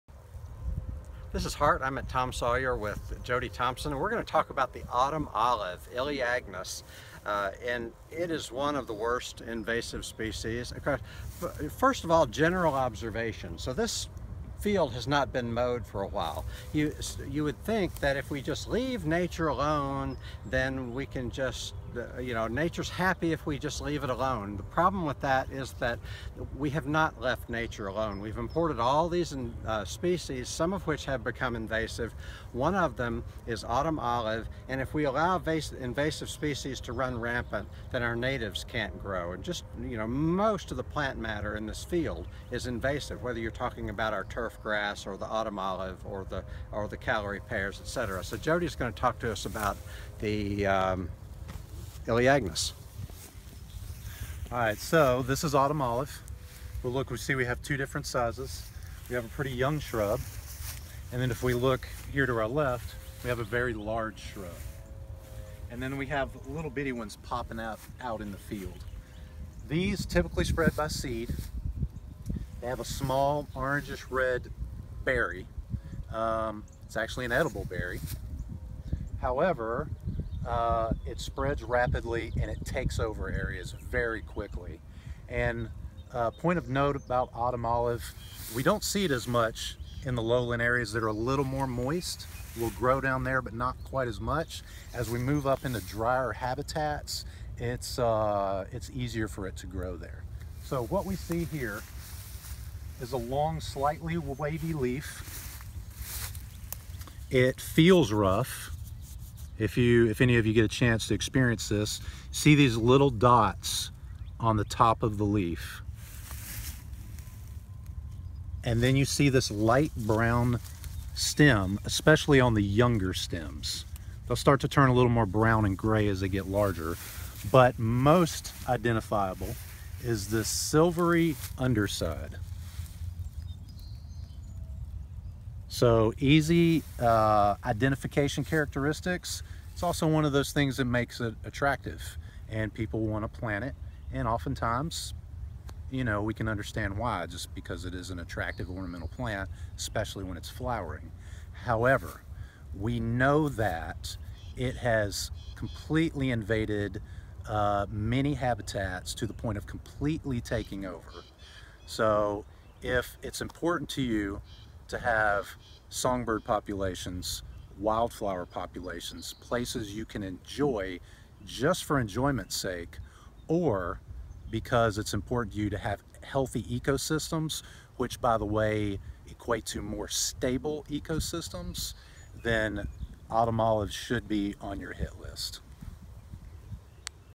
Autumn Olive (elaeagnus), an invasive bush
Autumn Olive is an attractive exotic species that unfortunately has escaped captivity and is running rampant over much of the eastern United States. Here is how to identify this species.